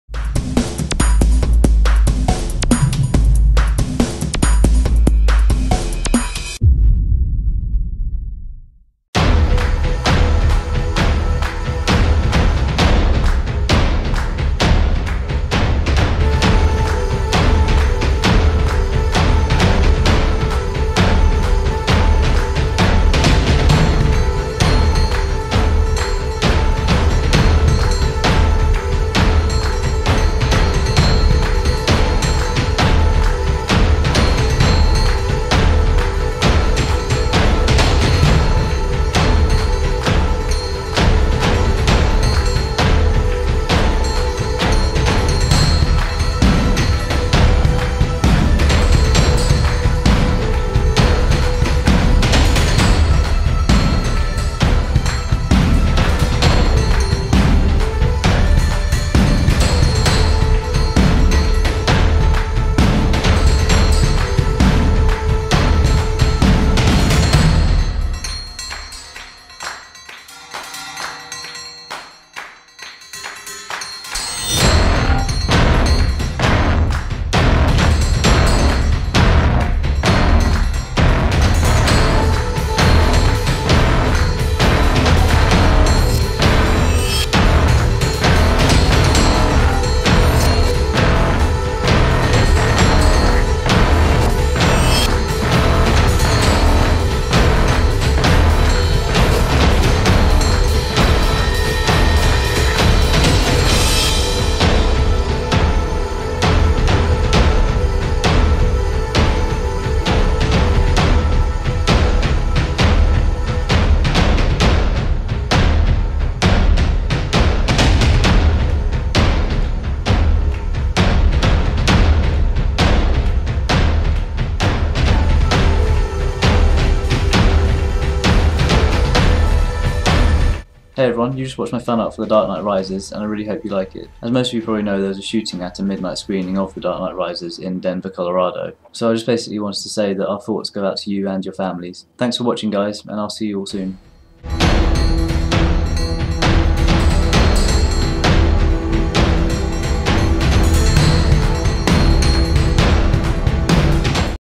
The Dark Knight Rises Fan Art - Drawing
Hey guys! My latest fan art, decided to try "The Dark Knight Rises" (TDKR) for the release.

I post regular videos to YouTube of my fan-art paintings and sketches in a time-lapse format.

I wanted to get a video out for the release of "The Dark Knight Rises" TDKR. I wasn't going to add a vlog but felt I had to say something about the Dark Knight Rises midnight release shootings in Denver, Colorado.

I own all rights to the content in this video.